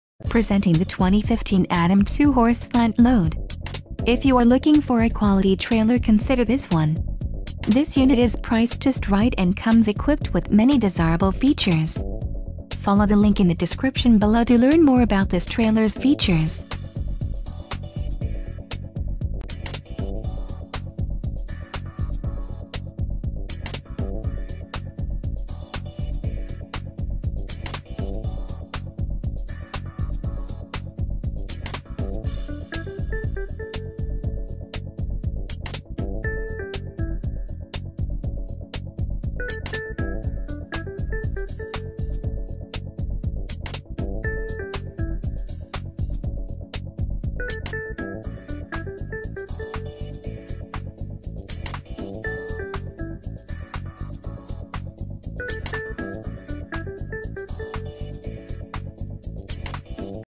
2015 ADAM 2-Horse Slant Load - Trailers of the East Coas...
" Barn Doors, V-Nose, 2 - 3500 lb. Axles, Head Divider, Dressing Room, Dome Lights, Running Boards, Spare Tire, Bridle Hooks, Drop Feed Windows, Padded Divider"